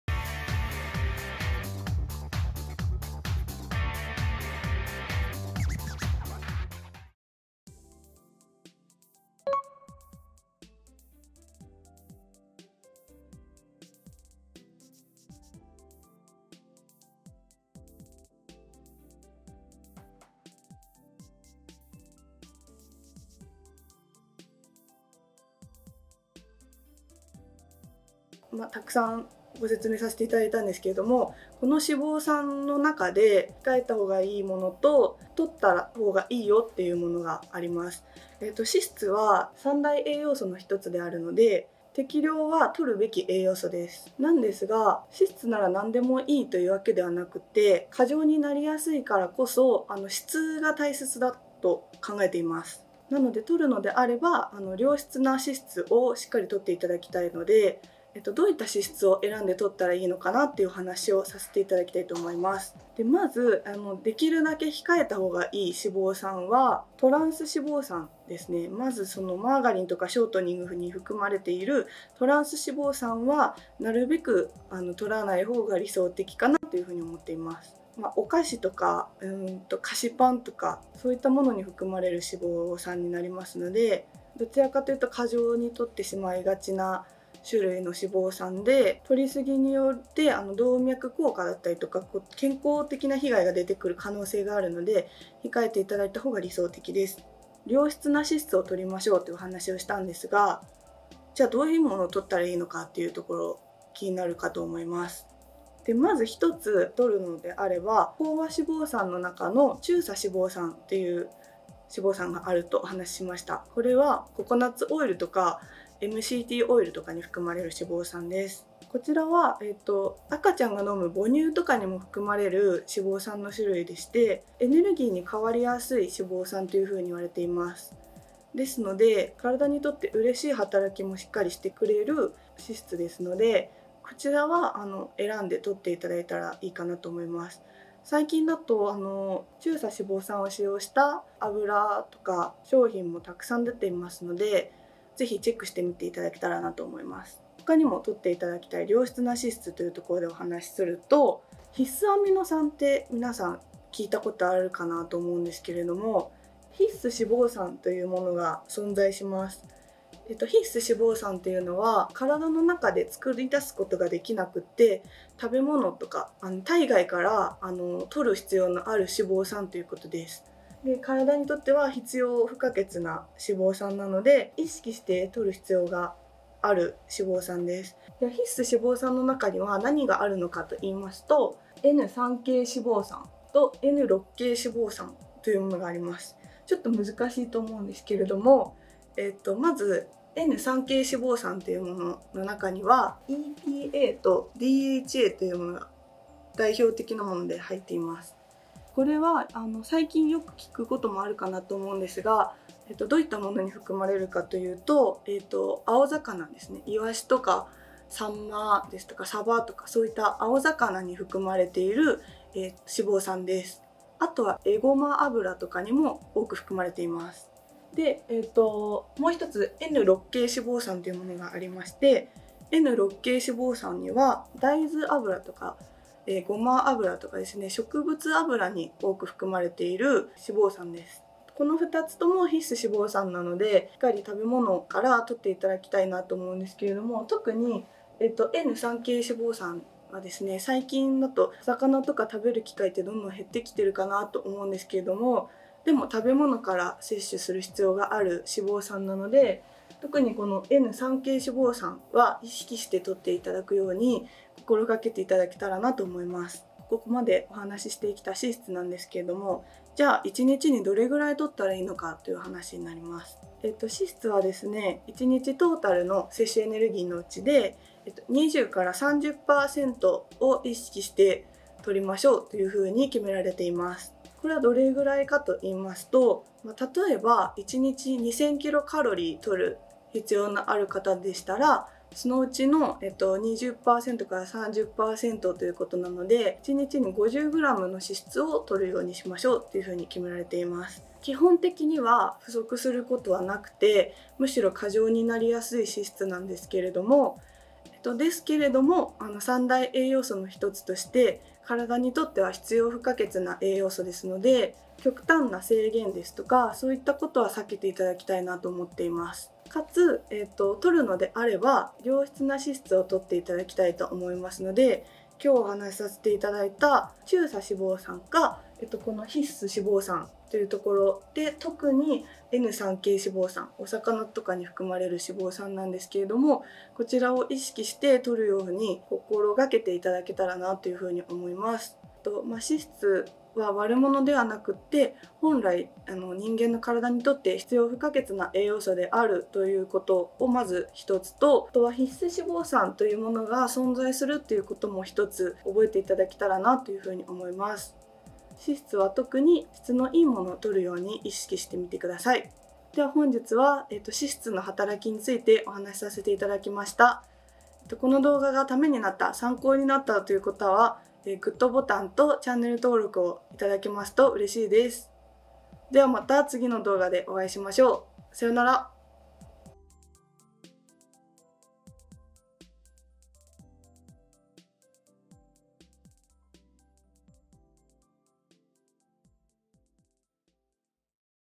【脂質】避けるべき脂質と摂るべき脂質「ビーレジェンド管理栄養士 堀のプチ栄養講座」【ビーレジェンド プロテイン】
ビーレジェンド管理栄養士 堀のプチ栄養講座
「摂るべき脂質」と「控えるべき脂質」について
なぜ摂る必要があるのか、どの脂質を控えたほうがいいのかなど詳しく解説します。